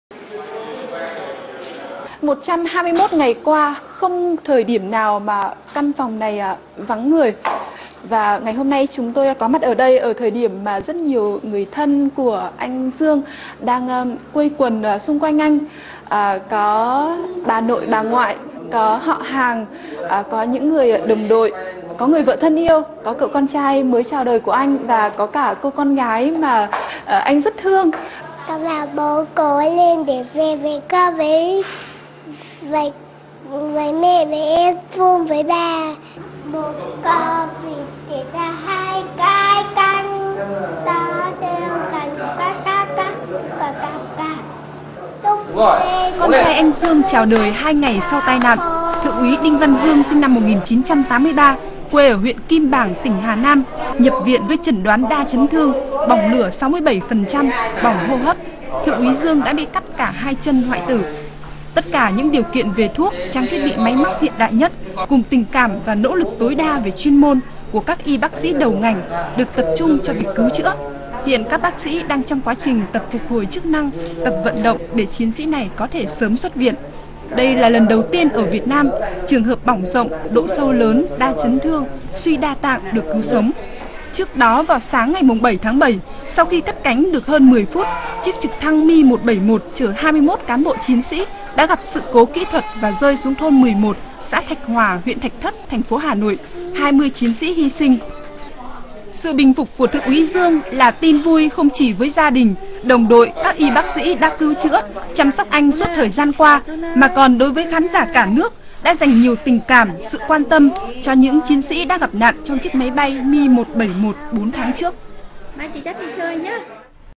Người duy nhất sống sót vụ trực thăng rơi đã nói chuyện.
Trung úy Đinh Văn Dương - người duy nhất còn sống sót trong vụ rơi trực thăng Mi171 tại Hòa Lạc ngày 7/7 vừa qua - đã tỉnh táo, thoát cơn nguy kịch, có thể nói chuyện được